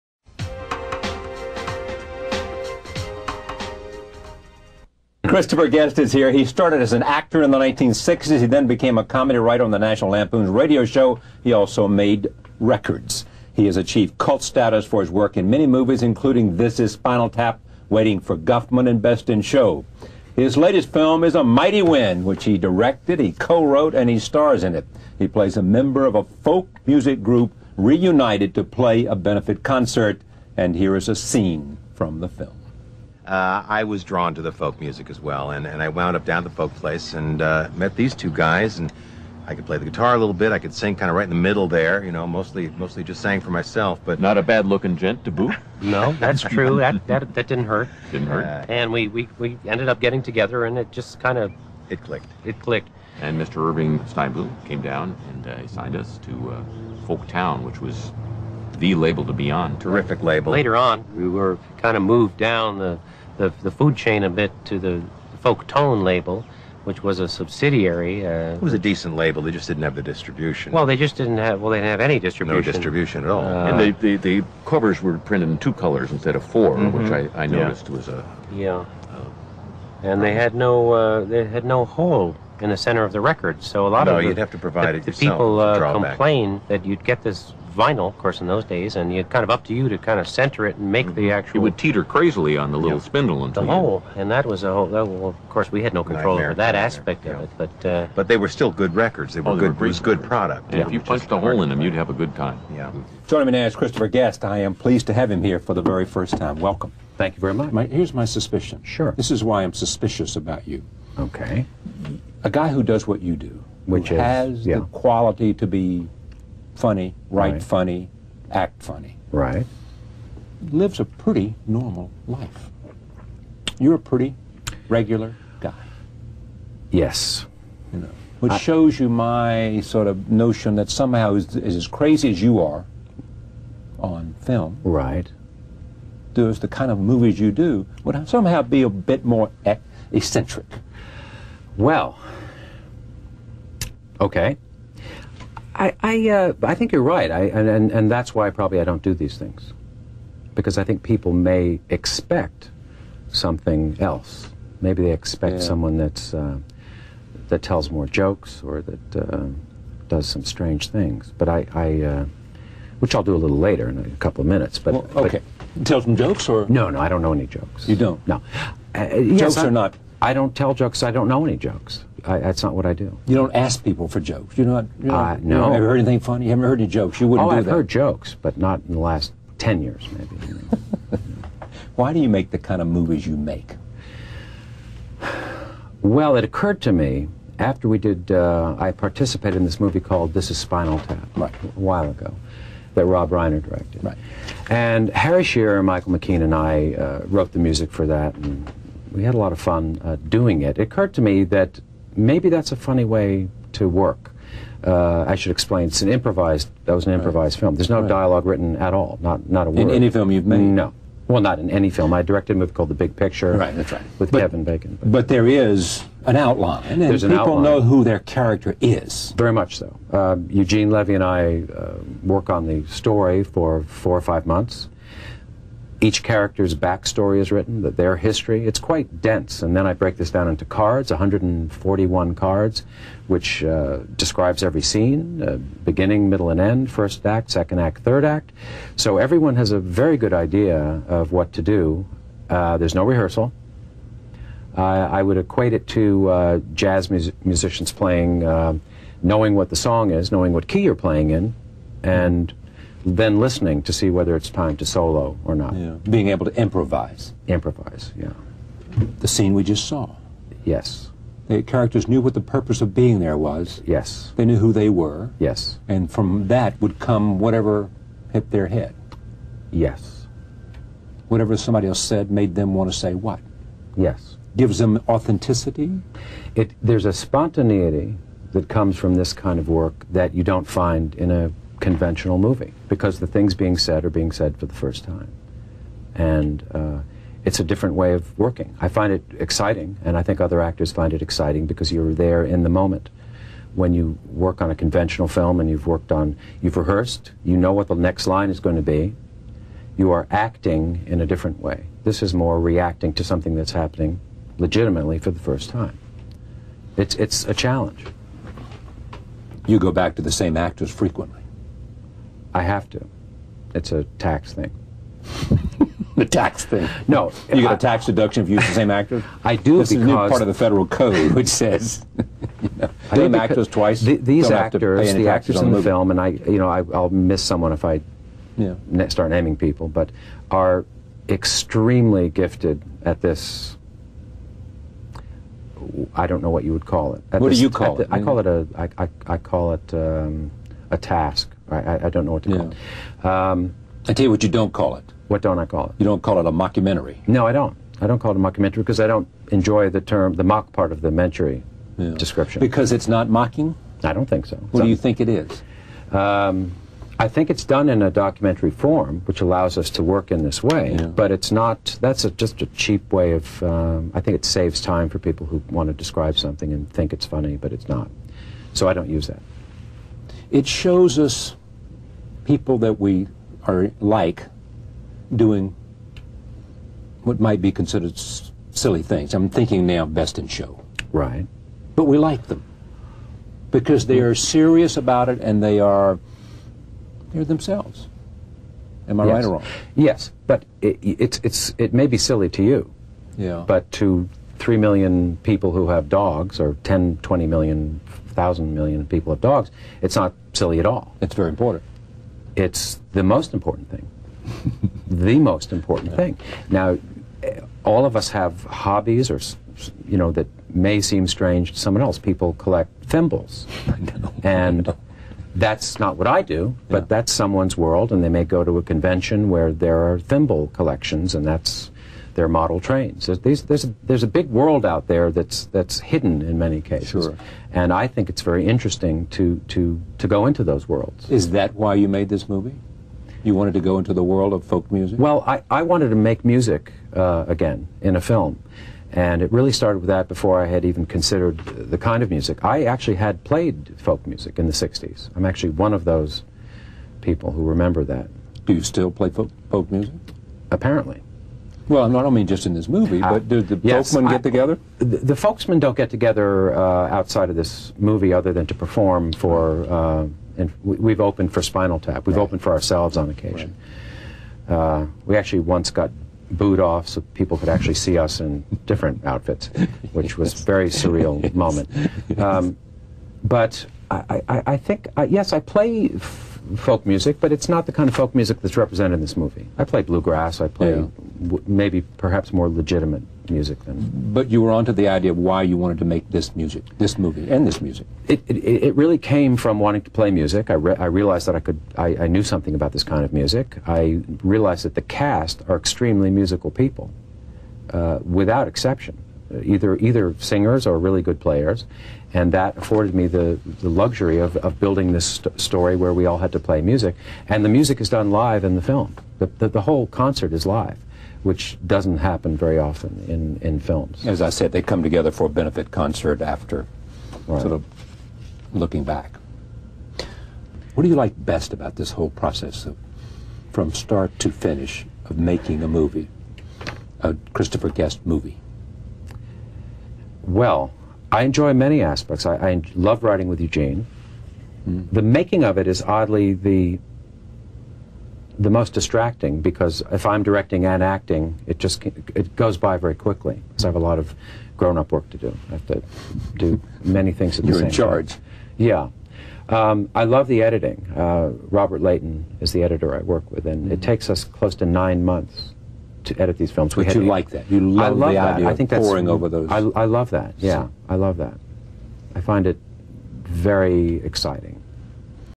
Chris Guest shmoozes about The Folksmen
CHRISTOPHER GUEST ON THE FOLKSMEN: "We've opened for Spinal Tap. We've opened for ourselves on occasion. We actually once got booed off. So that people could actually see us in different outfits. Which was a very surreal moment." [Christopher Guest]